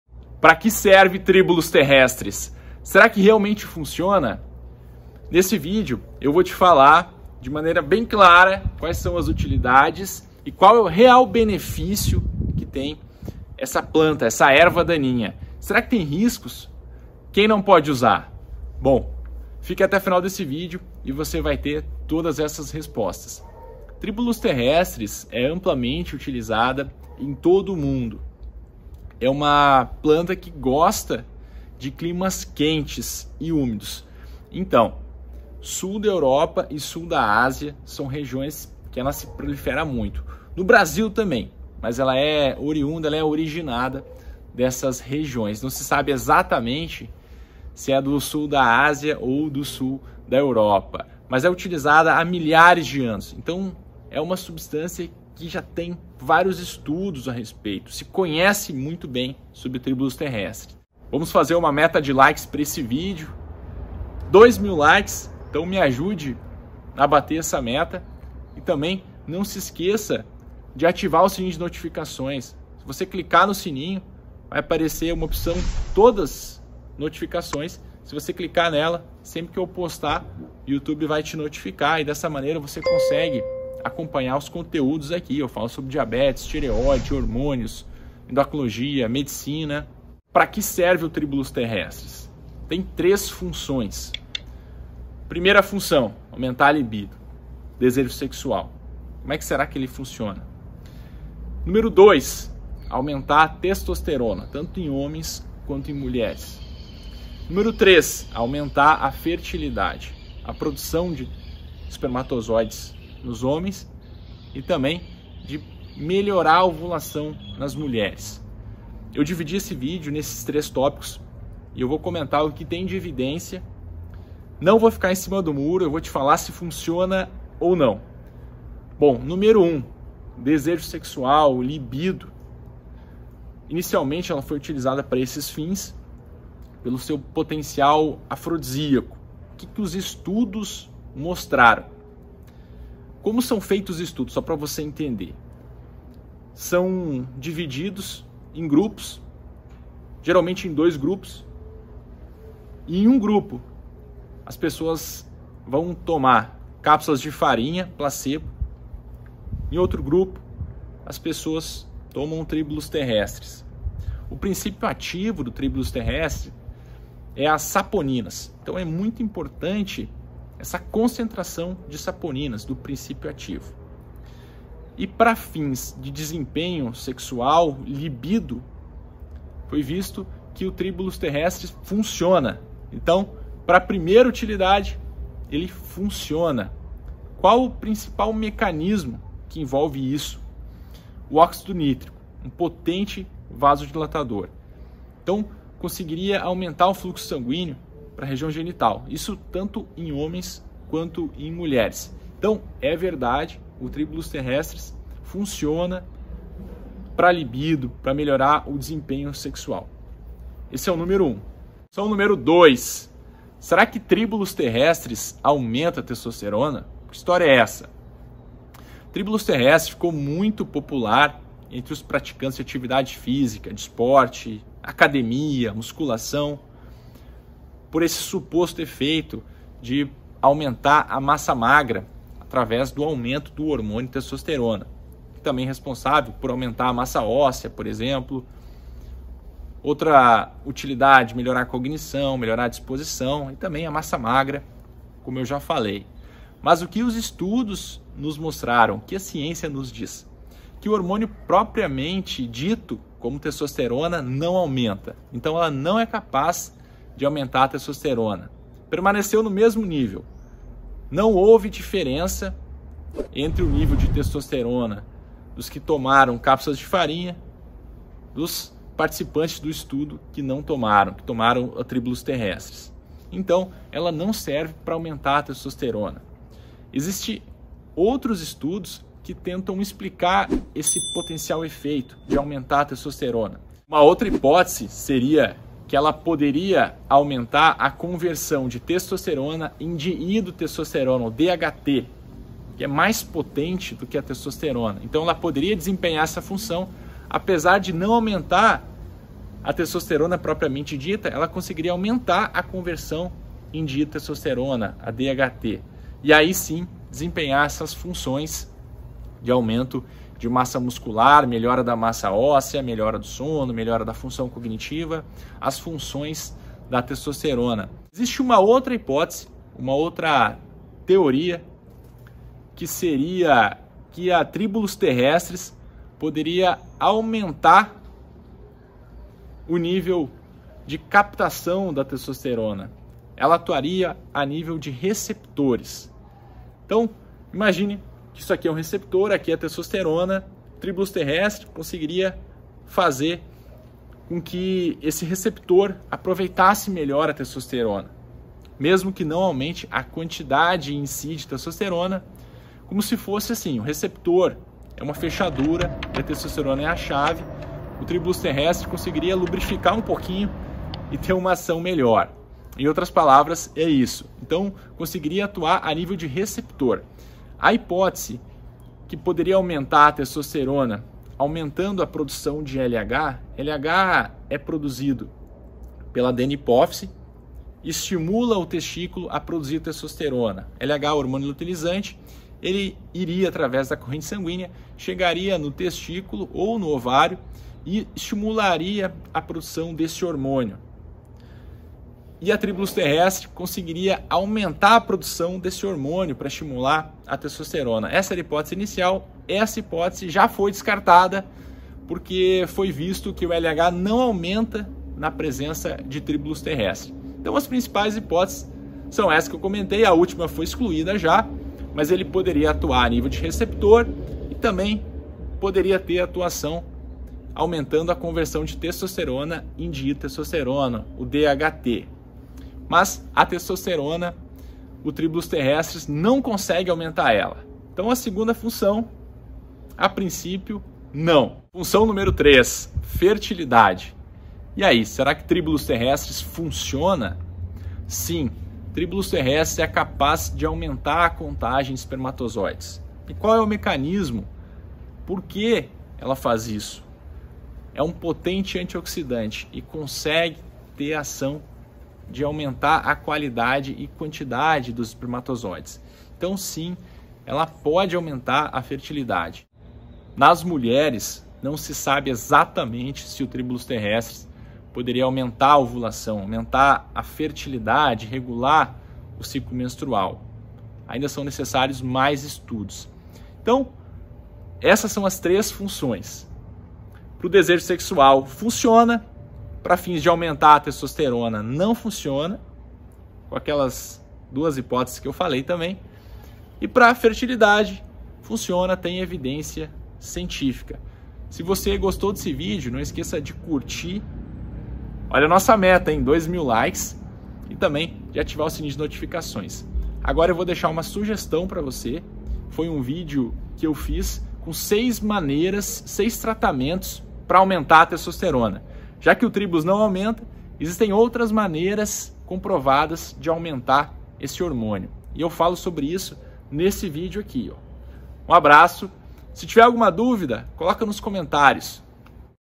TRIBULUS TERRESTRIS - Para Que Serve? Funciona Mesmo?
Tribulus Terrestris para que serve ? Funciona mesmo? Quais os benefícios de Tribulus Terrestris? Para que serve? Tudo sobre o uso de Tribullus Terrestris com evidências científicas. Será que aumenta a testosterona? Fertilidade? Tem efeitos colaterais? Riscos?

💥Equipamentos que uso: (Gravo pelo celular, não uso filmadora/câmera profissional)
Iphone 13 Pro
MacBook Air
Tripé ring light com bluetooth